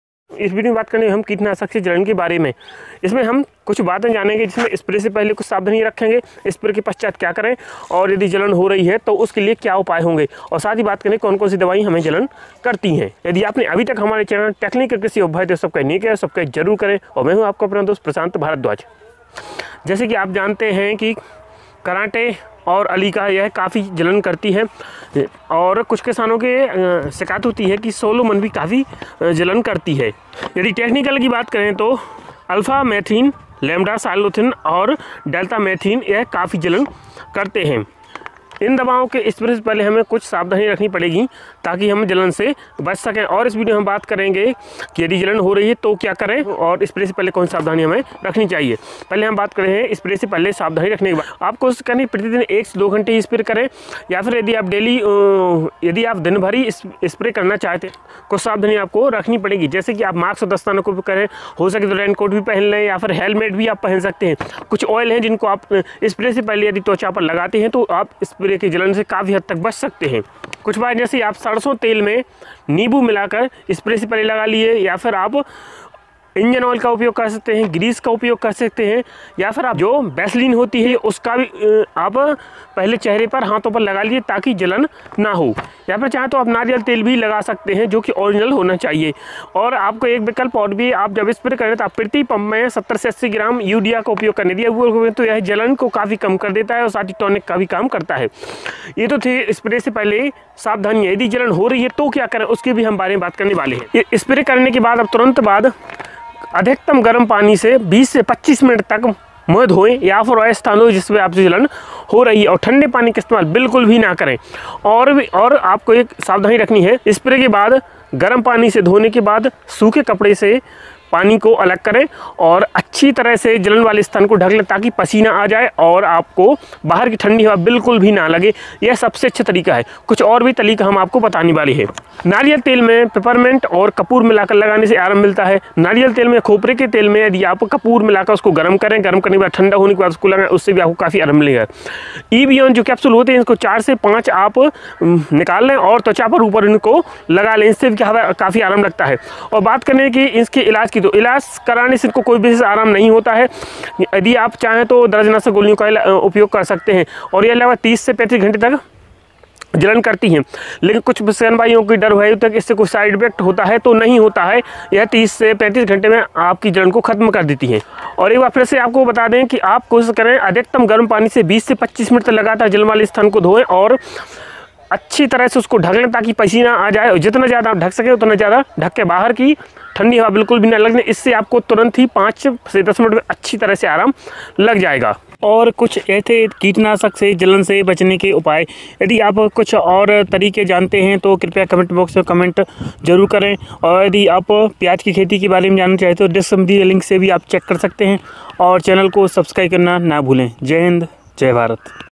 कीटनाशक के जलन से बचने के उपाय Ways to avoid pesticide burnsकृषि दवाओं खुजली से बचने के उपाय part2
इस वीडियो में हम कीटनाशक से होने वाले जलन के बारे में जानेंगे कुछ कीटनाशक जैसे कराटे अलिका सोलोमन, यह यदि त्वचा पर लग जाये तो काफी जलन करती है टेक्निकल की बात करे तो alphametrin  lembdacyhlothrin and deltamethrin जलन करते है इन से होने वाले जलन से बचने के लिए माक्स और दस्ताने का उपयोग करे हेलमेट भी पहन सकते है कुछ तेल जिनका स्प्रै से पहले त्वचा पर लगाने से कीटनाशक के जलन से बच सकते है  जैसे सरसो तेल में नीबू मिलाकर त्वचा पर लगाए इंजन आयल, ग्रीस, वेसलीन, नारियल तेल ,आदि भी लगा सकते है यदि  कीटनाशक के साथ घोल में १०० ग्राम यूरिया मिलाकर स्प्रै करे तो भी जलन काम होती है साथ यह टोनिक का काम भी करता है यदि जलन हो रही है तो कुछ उपाय कर सकते है स्प्रै  के बाद अधिकतम गर्म पानी जितना आप सहन कर सकते है उतने गर्म पानी से २०-२५ मिनिट तक त्वचा साफ करे कपडे का उपयोग न करे ठन्डे पानी का उपयोग न करे और जलन बाले स्थान को अच्छी तरह ढके ताकि बाहरी ठंडी नमी युक्त हवा न लगे बल्कि पसीना आ जाये इस से ५ मिनिट में ही आराम लग जाता है या फिर ४-५ evon केप्सूल त्वचा पर लगा ले या फिर नारियल तेल में कपूर मिलकर गर्म करे ठंडा होने लगाए या फिर नारियल तेल में पिपरमेंट  लगाये कीटनाशक से ३०-३५ घंटे तक जलन होती है लेकिन कोई दीर्घकालीन दुष्प्रभाव नहीं होते बिना इलाज किय भी आराम लग जाता है
In this video we will learn about the irritation caused by insecticides, some insecticides like karate alika solomon, if applied on the skin, it causes a lot of irritation. Talking about technical, alphametrin, lembdacyhlothrin and deltamethrin irritate, avoid irritation caused by these. Use masks and gloves for use, can also wear helmets, some oils that can be applied on the skin before spray can avoid insecticide burns, such as engine oil, grease, Vaseline, coconut oil, etc. You can also apply if you spray by mixing 100 grams of urea in the solution with insecticide, it also works as a tonic, if there is a burning sensation, then you can take some measures after spray, as much as you can tolerate. You can clean the skin with hot water for 20-25 minutes, do not use clothes, do not use cold water and cover the burning area well so that the outside cold moist air does not come in but sweat comes from this for 5 minutes I feel comfortable or apply 4-5 evon capsules on the skin or warm coconut oil mixed with camphor. Let it cool down or apply peppermint in coconut oil. The insecticide causes irritation for 30-35 hours but without any long-term side effects, it gets relief even without treatment.